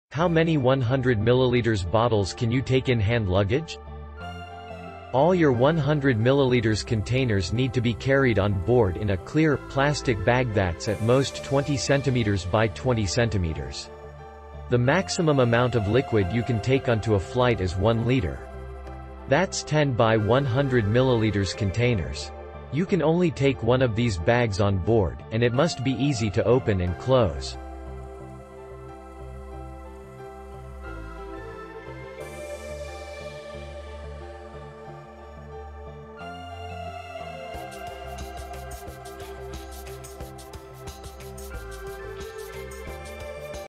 How many 100ml bottles can you take in hand luggage?
All your 100ml containers need to be carried on board in a clear, plastic bag that's at most 20cm x 20cm. The maximum amount of liquid you can take onto a flight is 1 litre. That's 10 x 100ml containers. You can only take one of these bags on board, and it must be easy to open and close.

Reference:
A Guide to Liquids in Hand Luggage on UK Flights | The London ...